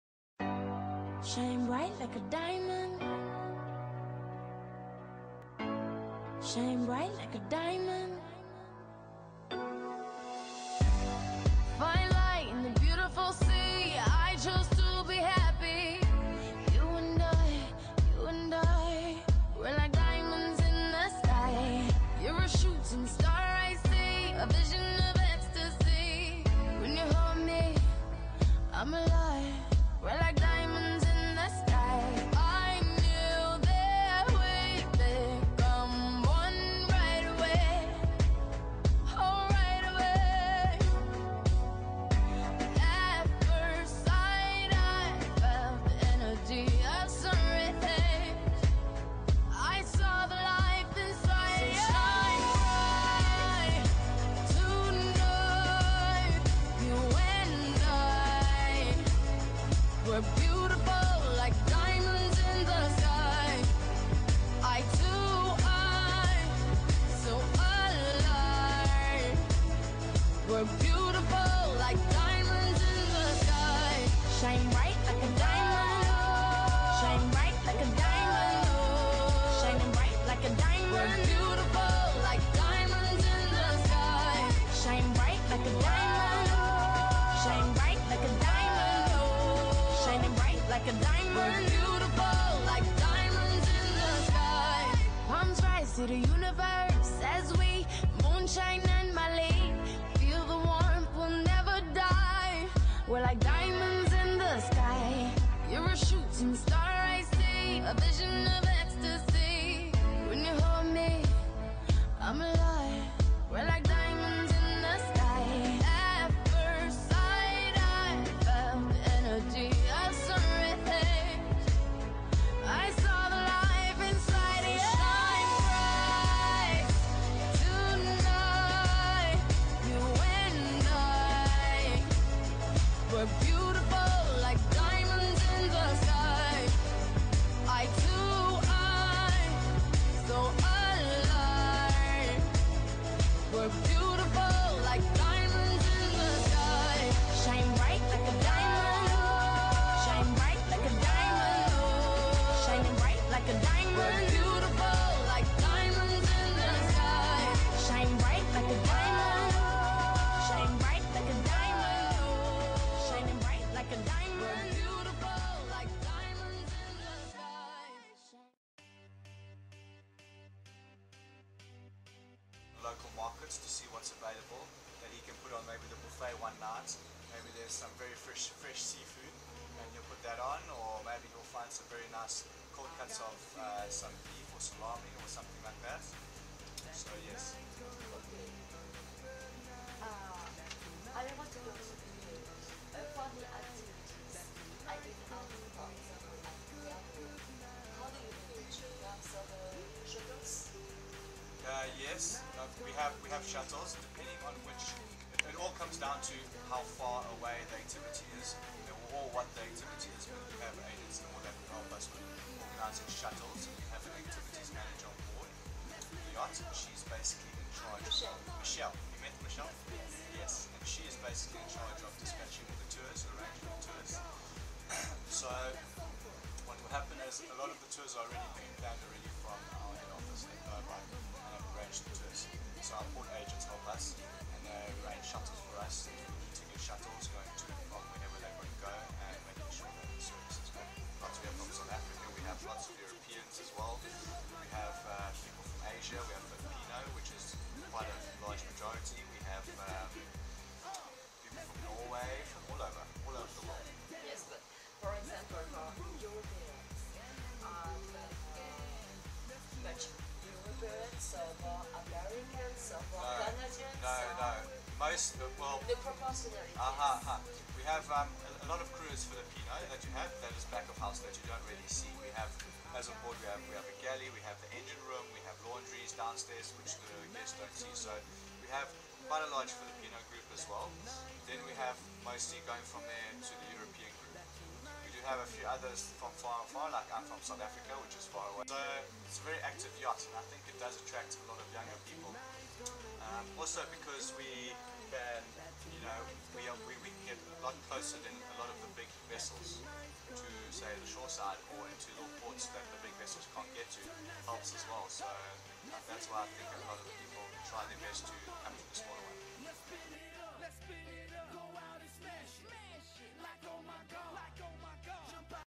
Escale aux Saintes pour le bateau de croisière "SeaDream 2"
Visite du paquebot de luxe et rencontre avec son directeur hôtelier.